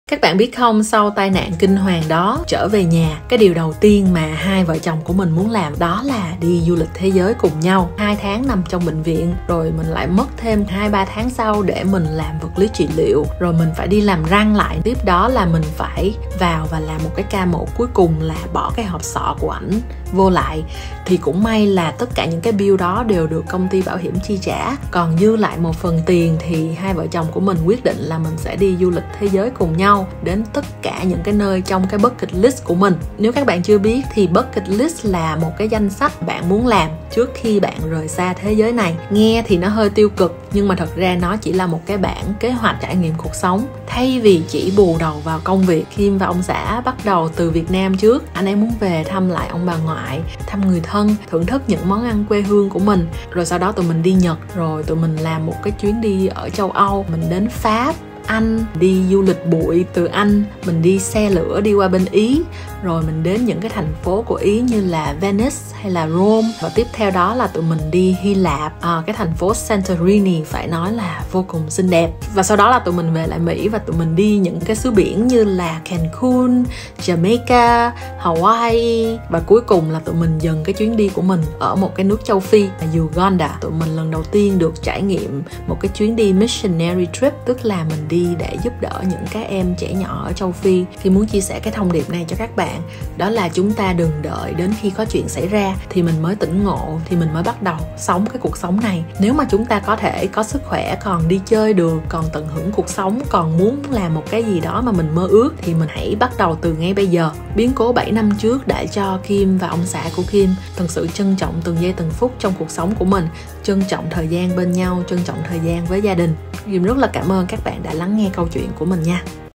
VIỆN PHÍ VÀ DU LỊCH | KIM VÀ CUỘC SỐNG MỸ
Chi phí phẫu thuật & điều trị của ông xã Kim sau tai nạn. Không chỉ là cái bill của bệnh viện, mà còn chi phí sinh hoạt suốt 6 tháng 2 vợ chồng không có đi làm.

Trong tình cảnh đó thì rất may Kim có được 2 khoản tiền: Thứ nhất là anh xã Kim có bảo hiểm tai nạn, nếu ko có BH thì nhiều ng phải bán nhà hoặc vay nợ để trả khoản tiền này.

Thứ 2, là trước khi cưới vợ chồng mình đã tiết kiệm được 1 số tiền dự phòng cho hôn nhân và tương lai. Lúc đó chẳng ai nghĩ là rồi anh sẽ gặp tai nạn hay gia đình có biến cố cả. Nhưng cứ làm được bao nhiêu thì đều dành dụm 1 cách đều đặn và kỷ luật. Thì cái số tiền này nó đủ để 6 tháng 2 người ko đi làm vẫn sống được.

Thì đó là những sự may mắn về mặt tài chính mà nhờ đó Kim và ông xã vượt qua được giai đoạn cực kỳ khó khăn, sau đó còn thể đi du lịch thế giới và rồi trở lại với nhịp sống bình thường như bây giờ. Kim cám ơn bạn đã ủng hộ câu chuyện của Kim nha.